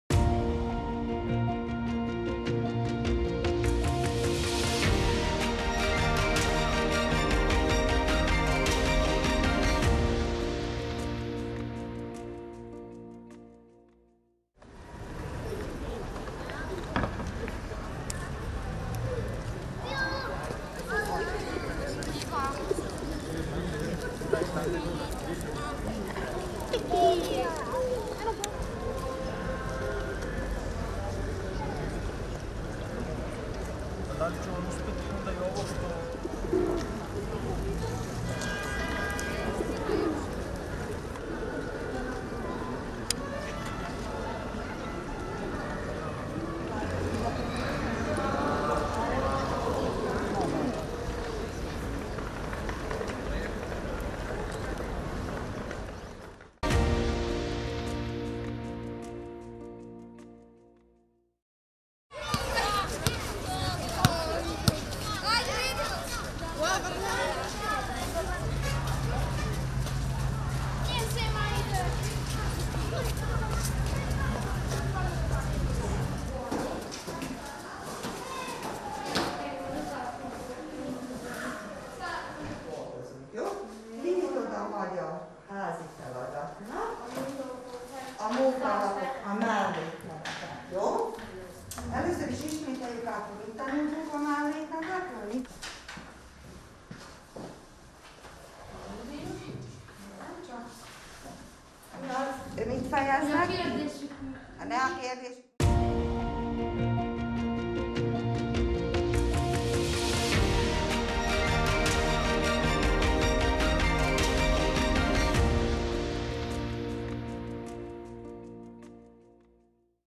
Kommentár nélkül - 2021.04.19.
Hírek, események képekben
- Szabadkai hétköznapok
- Délelőtti zsongás a Majsai Úti Általános Iskolában (Szabadka)